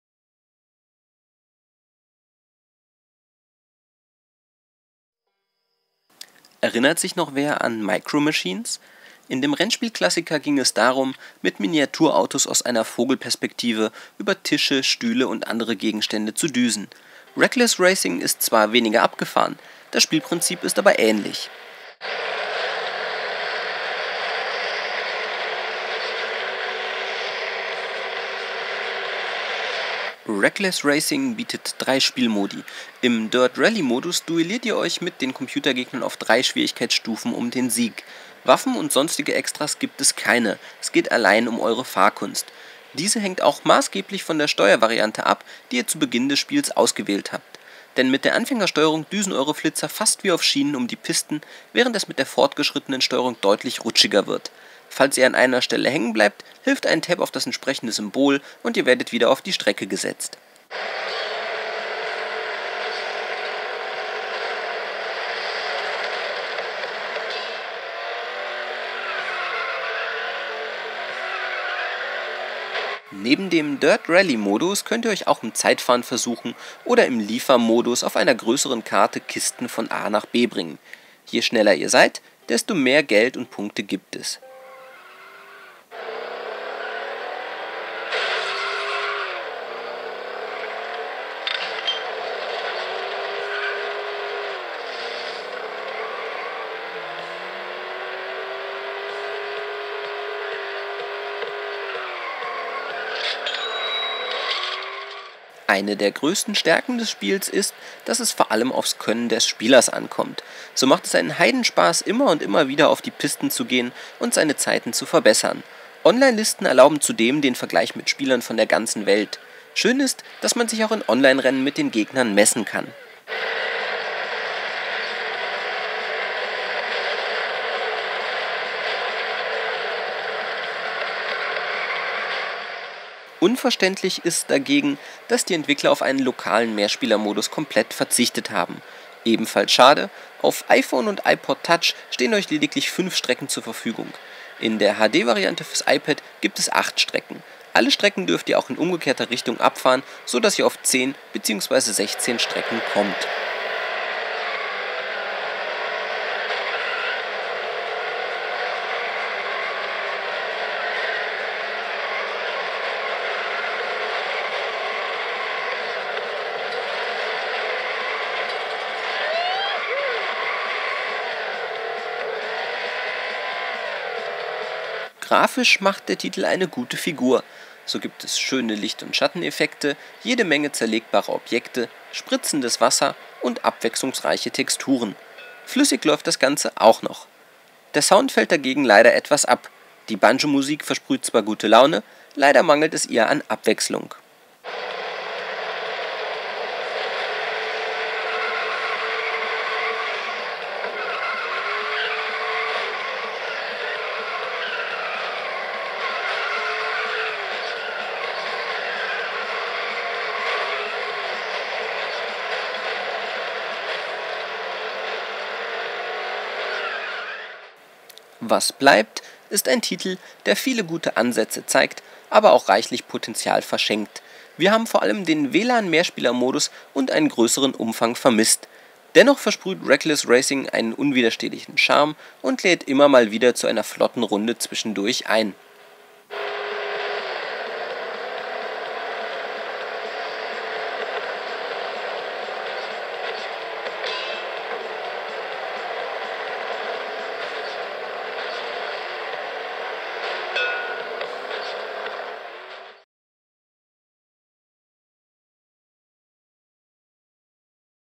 Reckless Racing - Test für iPhone / iPad / iPod Touch
Twitter Rennspiel auf den Spuren von Micro Machines mit reichlich verschenktem Potenzial. Alle Infos im Video-Test.